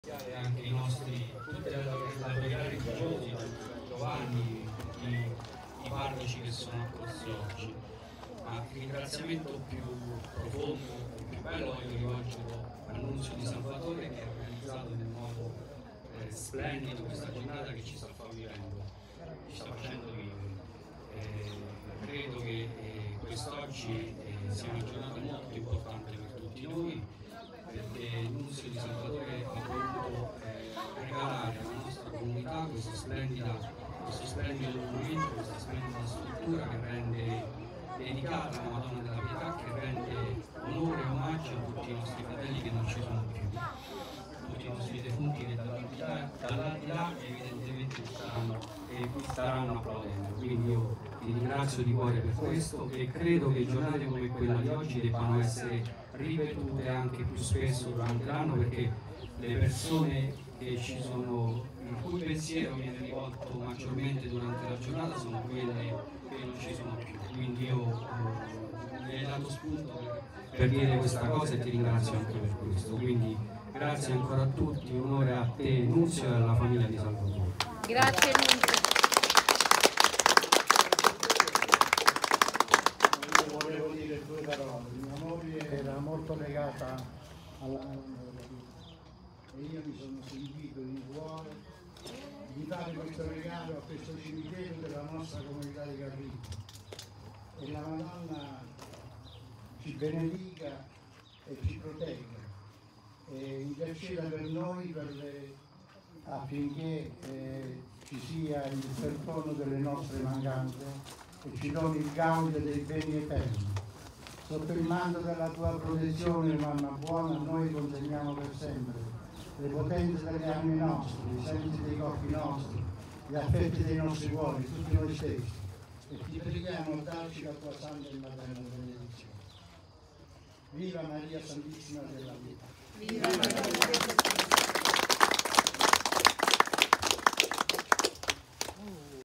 INAUGURAZIONE STATUA MADONNA DELLA PIETA'
Il ringraziamento del Sindaco